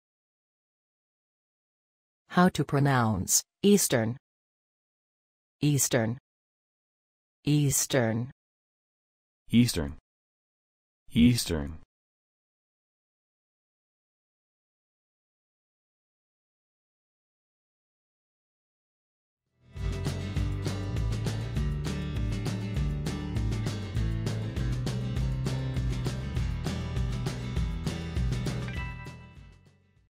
How to say Eastern
How to pronounce Eastern in English? Listen with us.
What is the correct pronunciation of the word Eastern in everyday English? Speak as the Americans.
How to write the word Eastern? Watch and learn.

Learn how to pronounce Eastern.